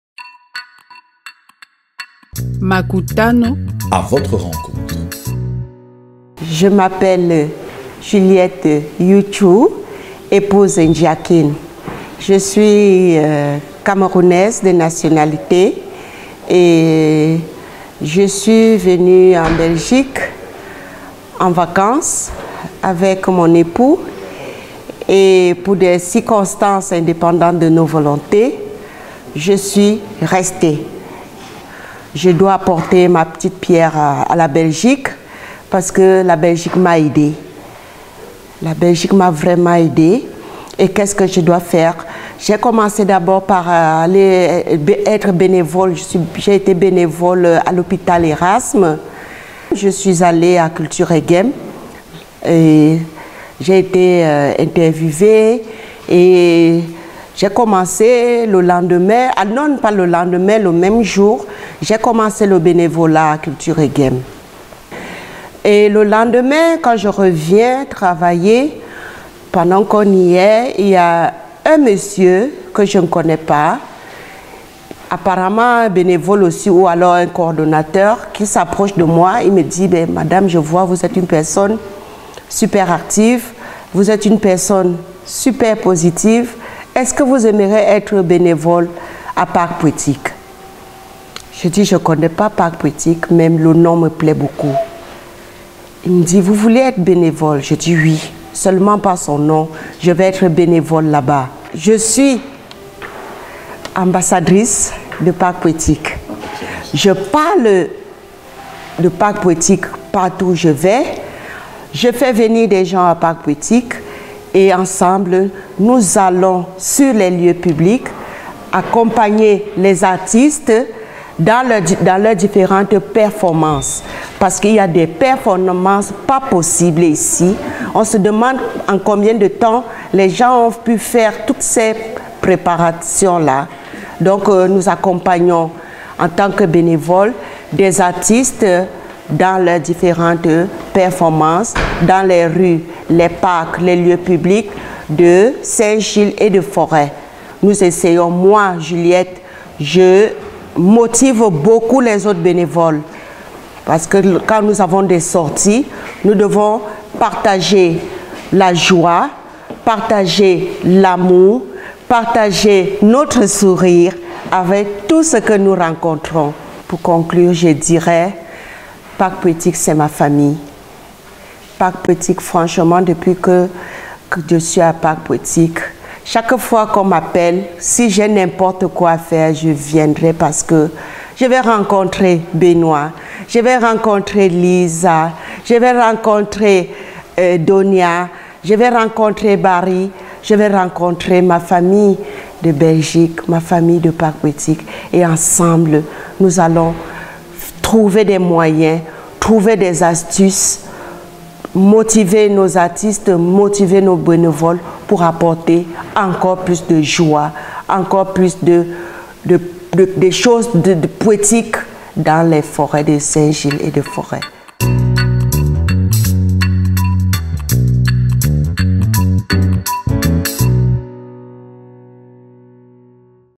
MAKUTANO: Juliette Youtchou
C'est la mise en perspective en 5 minutes d'un citoyen de la communauté africaine qui se valorise par son emploi ou tout autre activité justifiant ses revenus.